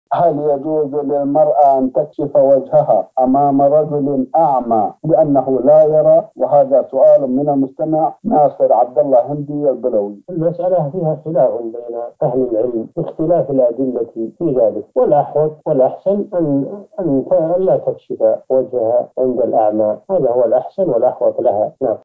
هل يجوز للمرأة أن تكشف وجهها أمام رجلٍ أعمى؟ الشيخ صالح الفوزان
نص السؤال: هل يجوز للمرأة أن تكشف وجهها أمام رجلٍ أعمى لأنه لا يرى؟
السيرة الذاتية :
للاشتراك في قناة الشيخ صالح الفوزان

رئيس الفريق
لمشروع كبار العلماء